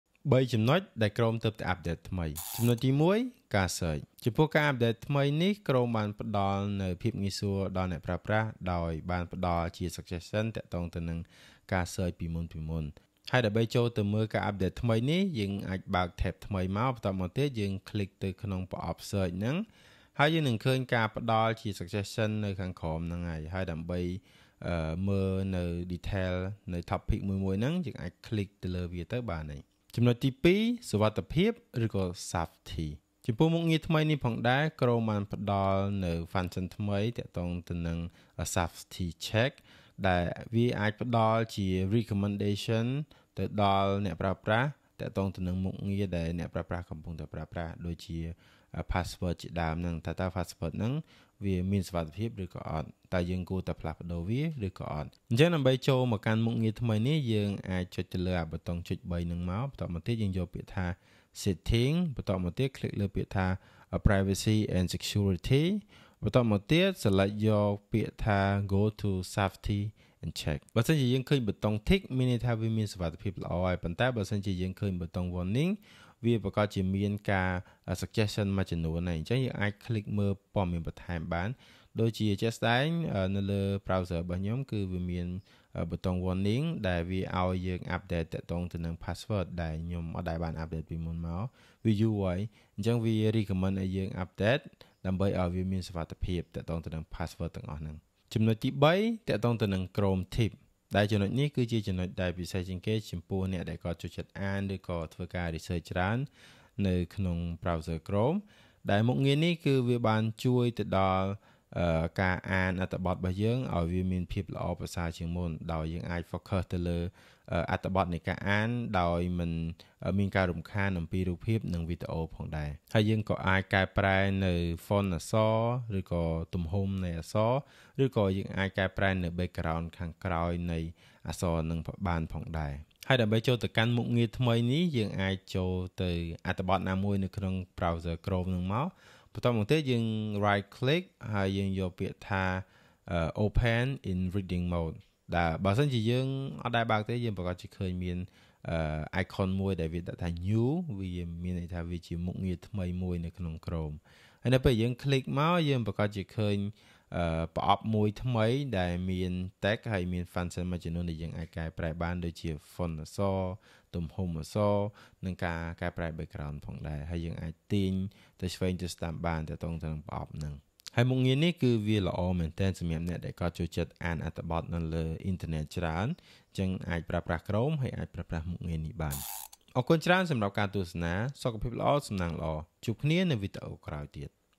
What's New in Chrome? | Top Features and Updates You Need to Know! | TechTips | Session 1 | Ep2
What's New in Chrome? | Top Features and Updates You Need to Know! TechTips | Session 1 | Ep2 | គន្លឹះបច្ចេកវិទ្យា

Discover the latest features and updates in Google Chrome! In this video, we'll explore the newest tools, enhancements, and changes that make browsing faster, safer, and more efficient. Whether you're a casual user or a tech enthusiast, stay up-to-date with Chrome's cutting-edge improvements and tips to make the most of your browsing experience. Don't miss out on the exciting innovations from the world's most popular web browser!

- Gear
1. Sony ZV-E10 with kid Lens
2. Microphone: PD200X